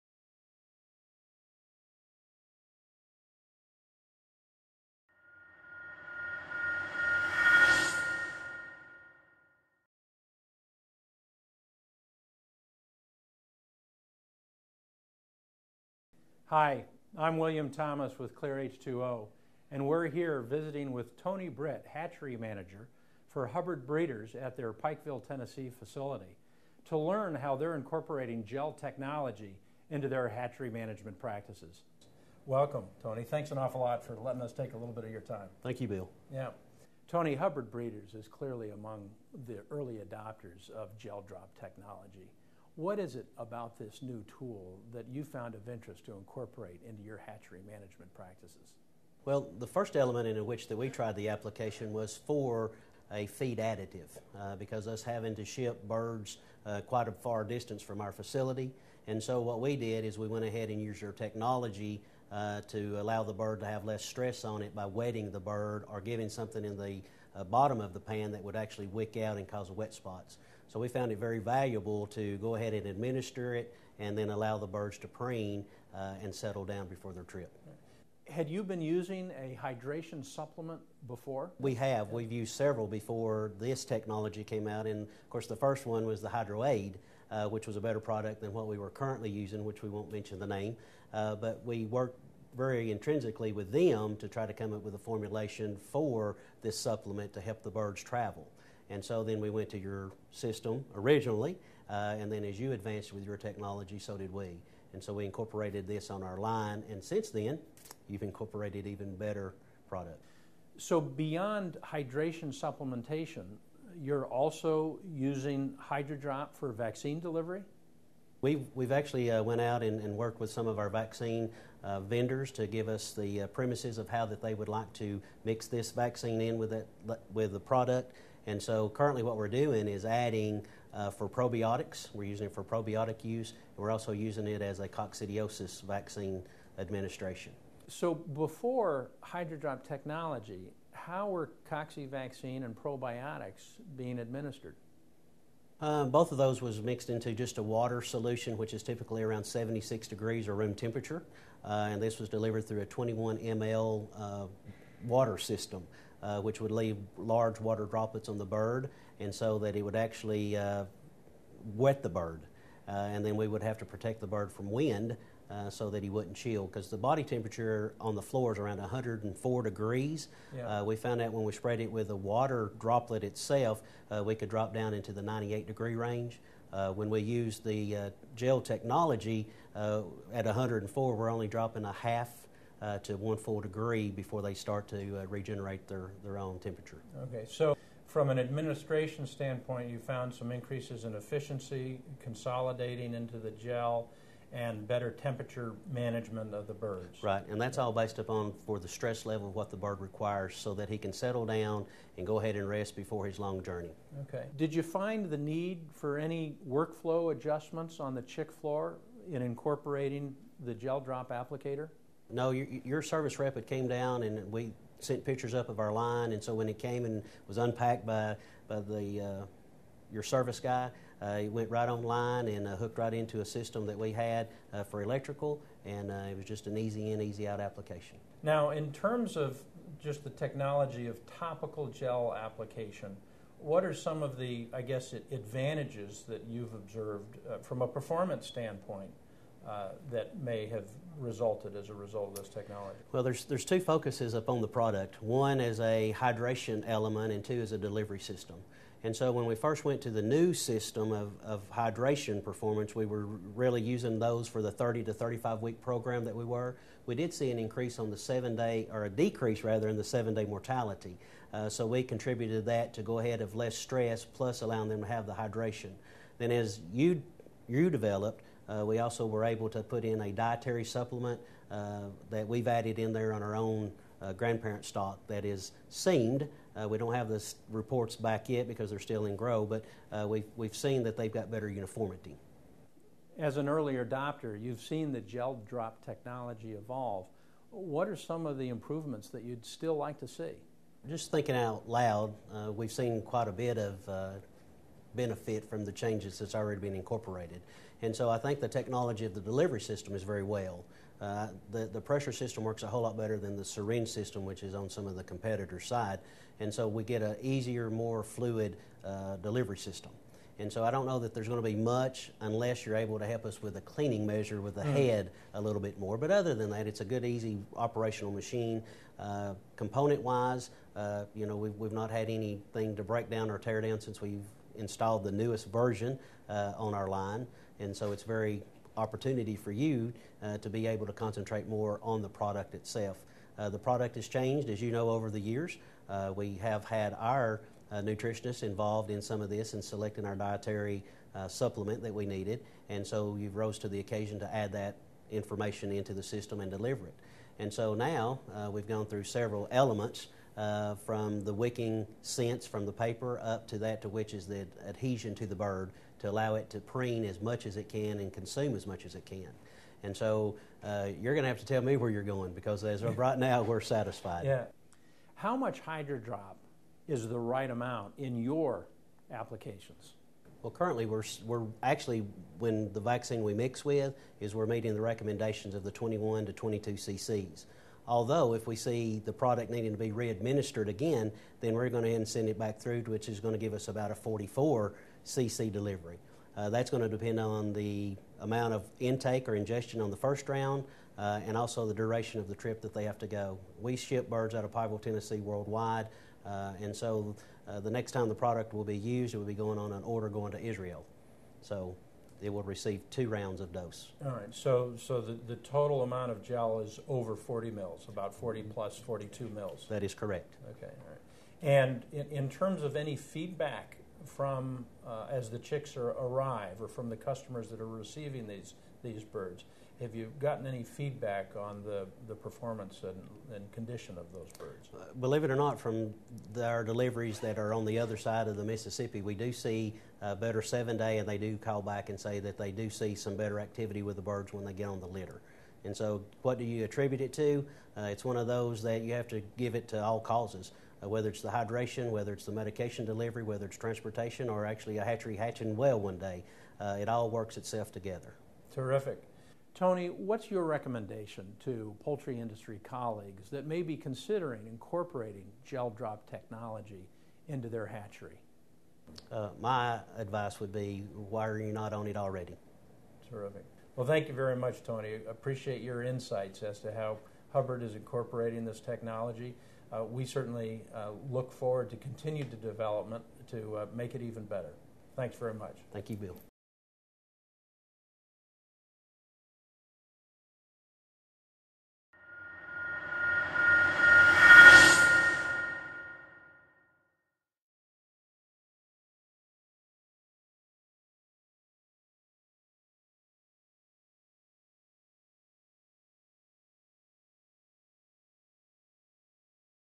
GelDrop® Technology in Broiler Breeder Operations
An Interview with Tony Britt, Hatchery Manager - Hubbard Breeders